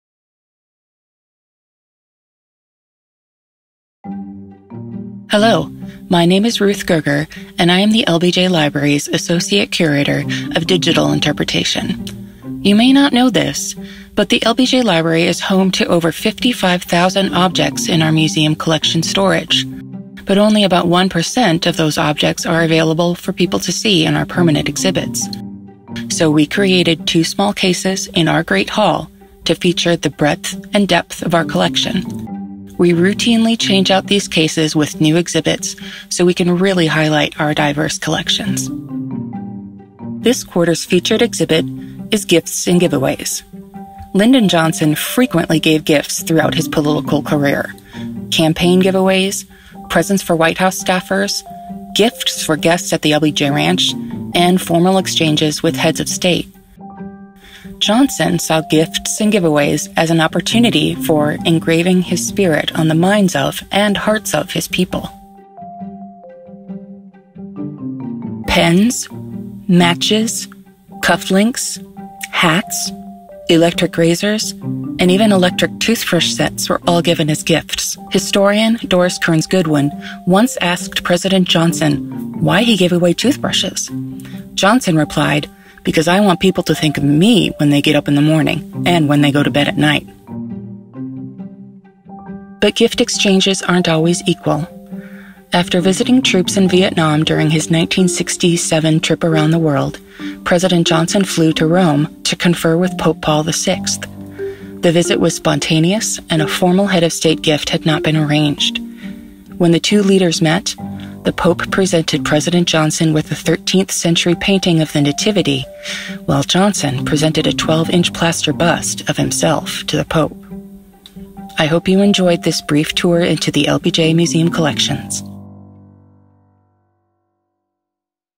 Virtual Walk-through: LBJ Gifts & Giveaways
Join Ruth Goerger, associate curator of digital interpretation, for a virtual walk-through of "Gifts & Giveaways," a temporary exhibition on view in the LBJ Presidential Library's Great Hall.

/// ABOUT
Lyndon Johnson frequently gave gifts throughout his political career: campaign giveaways, presents for White House staffers, gifts for guests at the LBJ Ranch, and formal exchanges with heads of state. Johnson saw gifts and giveaways as an opportunity for "engraving [his] spirit on the minds of and hearts of [his] people."

Pens, matches, cufflinks, hats, electric razors, and even electric toothbrush sets were all given as gifts. Historian Doris Kearns Goodwin once asked President Johnson why he gave away toothbrushes. Johnson replied, "Because I want people to think of me when they get up in the morning and when they go to bed at night."

Gift exchanges weren't always equal. After visiting troops in Vietnam during his 1967 trip around the world, President Johnson flew to Rome to confer with Pope Paul VI. The visit was spontaneous and a formal head of state gift had not been arranged. When the two leaders met, the Pope presented President Johnson with a 13th century painting of the Nativity, while Johnson presented a 12-inch plaster bust of himself to the Pope.